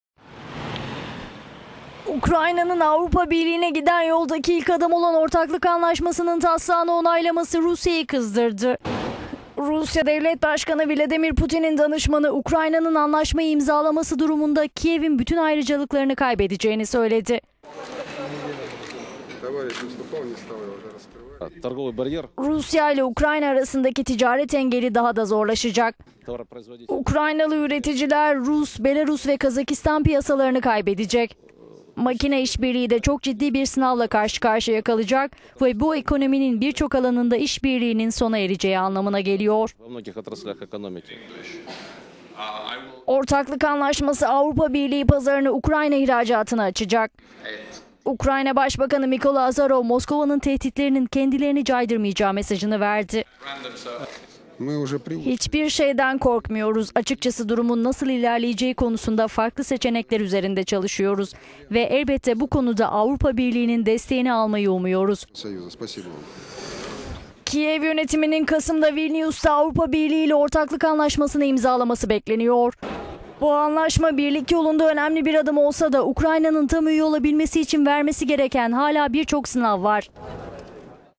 Ukrayna ile Rusya arasında Ortaklık Anlaşması gerginliği
Ukrayna'nın Avrupa Birliği'ne giden yoldaki ilk adım olan Ortaklık Anlaşması'nın...

Ukrayna'nın Avrupa Birliği'ne giden yoldaki ilk adım olan Ortaklık Anlaşması'nın taslağını onaylaması Rusya'yı kızdırdı. Ortaklık Anlaşması, Avrupa Birliği pazarını Ukrayna ihracatına açacak. Rusya ise Ukrayna'nın kendi öncülüğündeki Gümrük Birliği'ne katılmasını istiyor.

Rusya Devlet Başkanı Vladimir Putin'in danışmanı Ukrayna'nın söz konusu anlaşmayı imzalaması durumunda Kiev'in bütün ayrıcalıklarını kaybedeceğini belirtti: "Rusya ile Ukrayna arasındaki ticaret engeli daha da zorlaşacak. Ukraynalı üreticiler Rus, Belarus ve Kazakistan piyasalarını kaybedecek.

Ukrayna Başbakanı Mikola Azarov, Moskova'nın tehditlerinin kendilerini caydırmayacağı mesajını verdi: "Hiçbir şeyden korkmuyoruz. Açıkçası durumun nasıl ilerleyeceği konusunda farklı seçenekler üzerinde çalışıyoruz ve elbette bu konuda Avrupa Birliği'nin desteğini almayı umuyoruz."